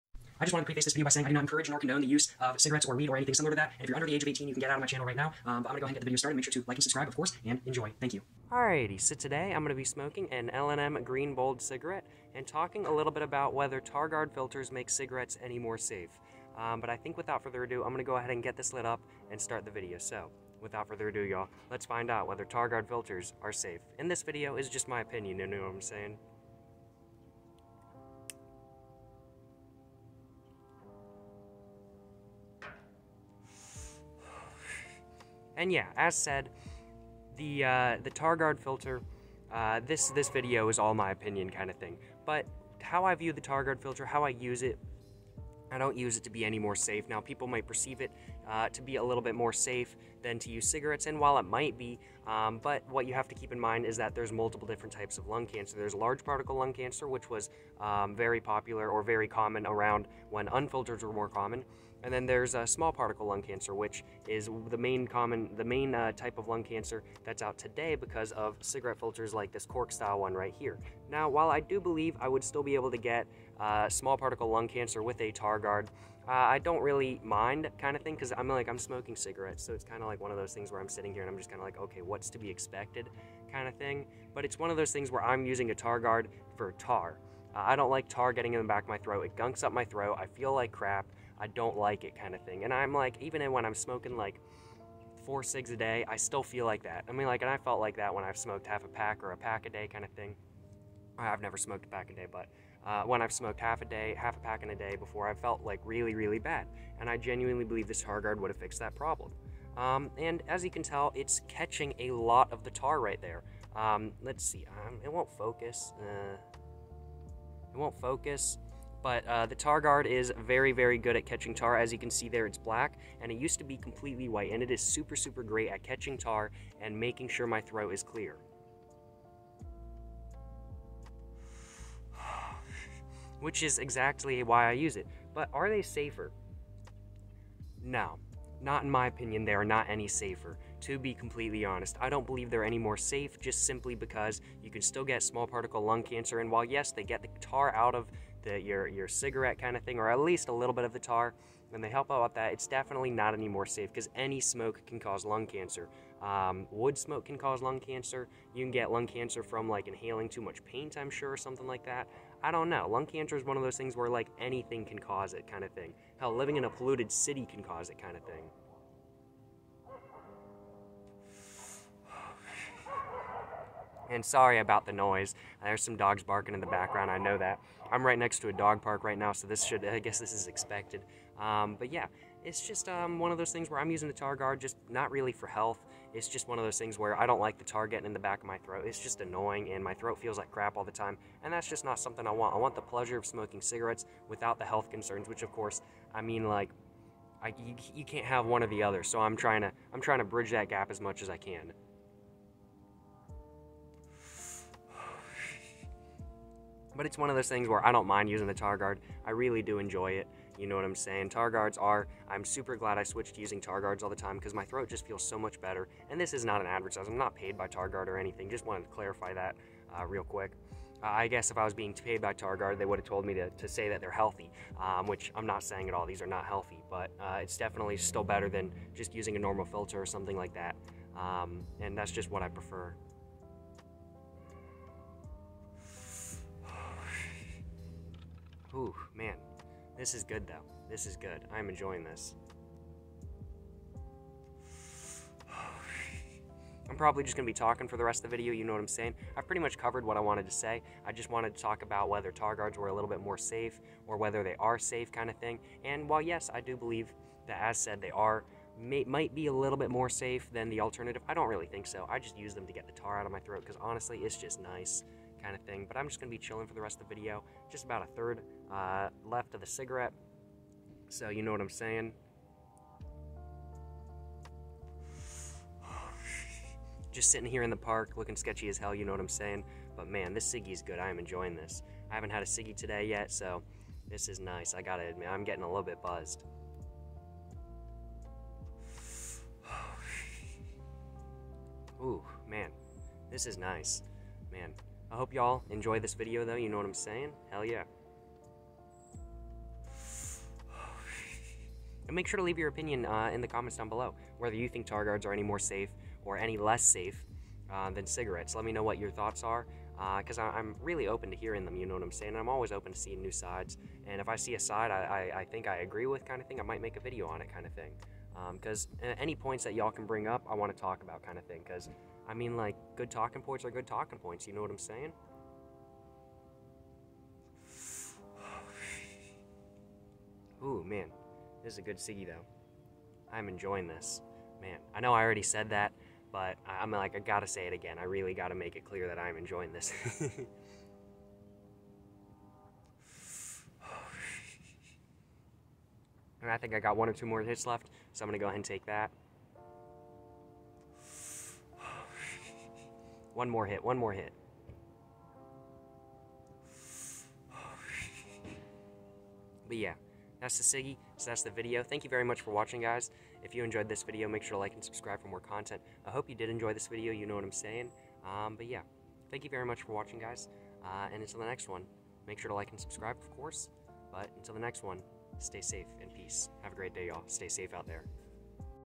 Do TarGuard Filters make Cigarettes Safe?
so, do targuard filters make cigarettes safe? well, it's really a matter of opinion. and today, i'm gonna talk about my opinion on targuards, and whether they make cigarettes any more safe! enjoy!

I do not encourage or condone the use of Tobacco or Tobacco Products.